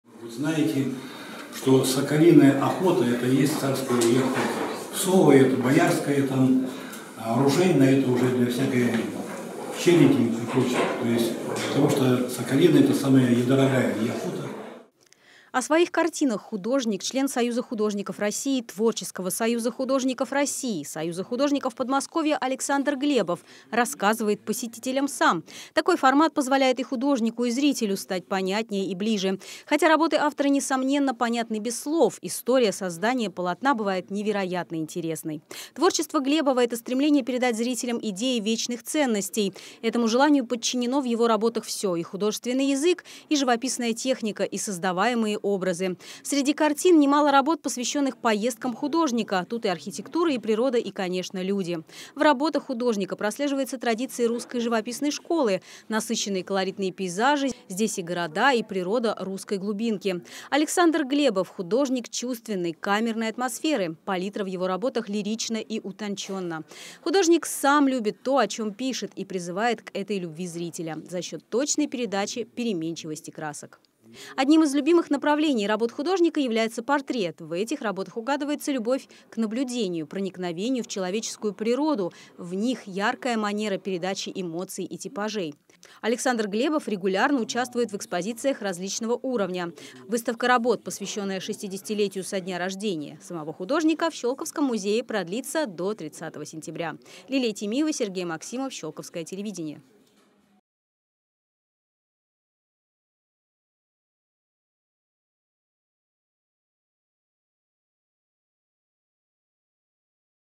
Выставка Александра Глебова представлена в галерее Щёлковского историко-художественного музея
Портрет, пейзаж, натюрморт, жанровая картина – всё это представлено на авторской выставке Александра Глебова в галерее Щёлковского историко-художественного музея. Живописные и графические работы, созданные в разное время – своеобразное подведение итогов определённого жизненного периода художника к его юбилею.